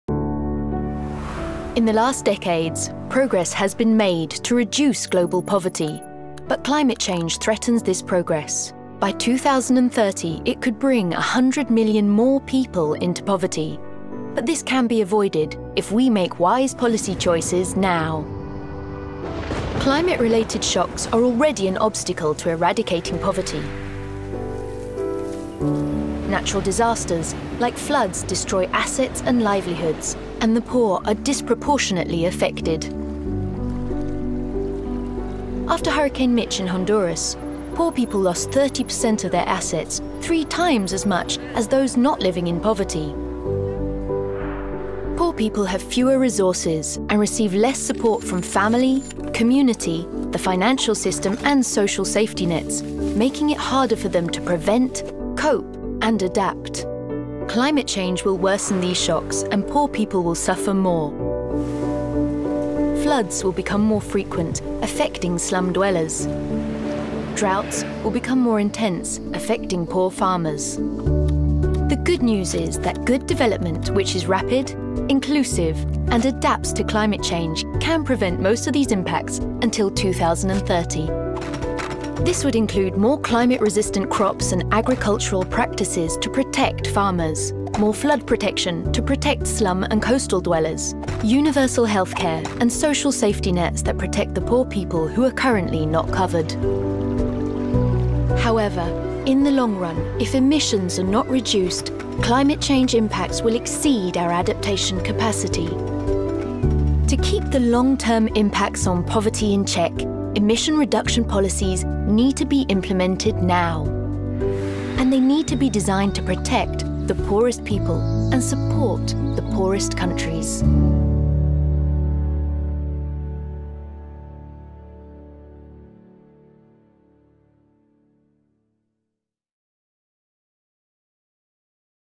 Cómo abordar los efectos del cambio climático en la pobreza
Los pobres ya tienen un alto riesgo de sufrir perturbaciones relacionadas con el clima, como por ejemplo la pérdida de cosechas debido a menores precipitaciones, alzas en los precios de los alimentos después de fenómenos meteorológicos extremos y una mayor incidencia de enfermedades tras olas de calor e inundaciones. Sin un desarrollo rápido, inclusivo e inteligente en relación con el clima y la aplicación de iniciativas para reducir las emisiones que protejan a los pobres, es posible que antes de 2030 haya otros 100 millones de personas en esa situación, en especial en África y Asia meridional.